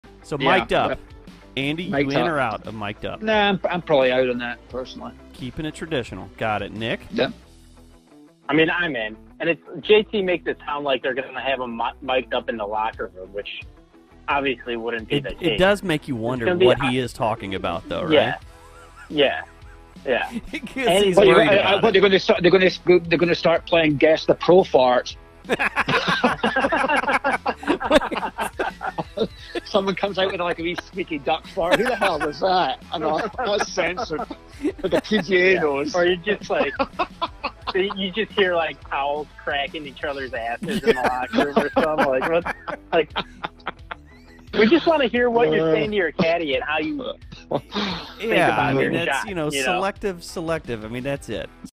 PGA Tour Mic'd Up ... Maybe | CART PATH ONLY Golf
The boys of @cartpathonlypod have differing opinions on the @pgatour Mic'd Up. To mic up or not to mic up, that's the question.

And if you're feeling bold, head over to TikTok and follow there -- @cartpathonly.